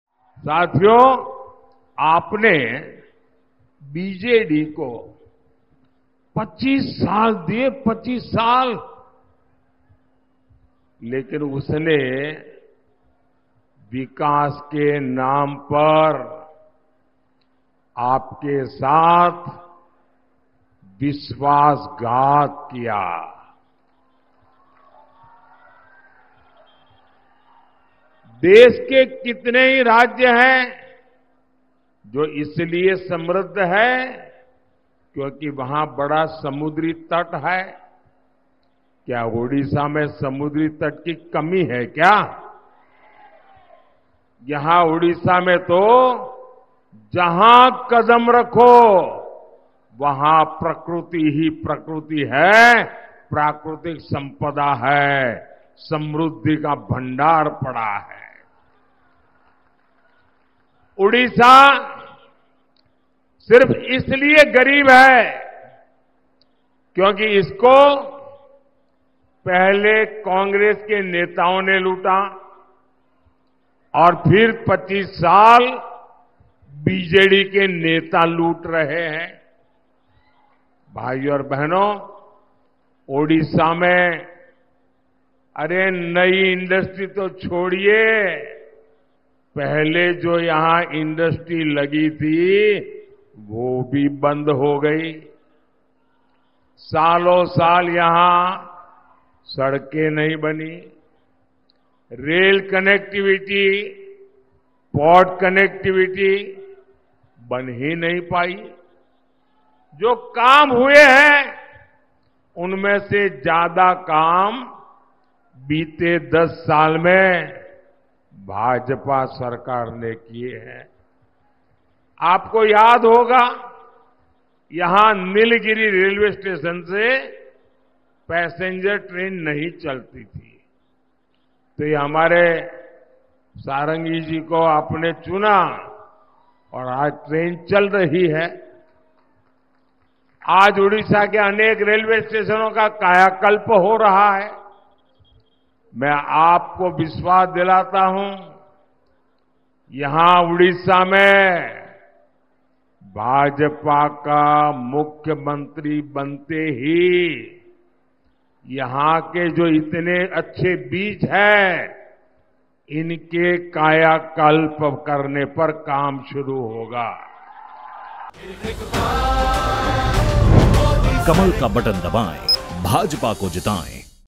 ओडिशा को पहले कांग्रेस के नेताओं ने लूटा 25 साल से बीजेडी के नेता लूट रहे हैं: PM Modi
ओडिशा सिर्फ इसलिए गरीब है क्योंकि इसे पहले कांग्रेस के नेताओं ने लूटा फिर 25 साल से बीजेडी के नेता लूट रहे हैं।

ओडिशा में पहले जो इंडस्ट्री लगी हुई थी वह भी बंद हो गई है। सड़कें नहीं बनीं, रेल, पोर्ट कनेक्टिविटी नहीं बनी। जो काम हुए हैं, उनमें से ज़्यादा काम बीते 10 साल में भाजपा सरकार ने किए हैं।

 - प्रधानमंत्री श्री नरेन्द्र मोदी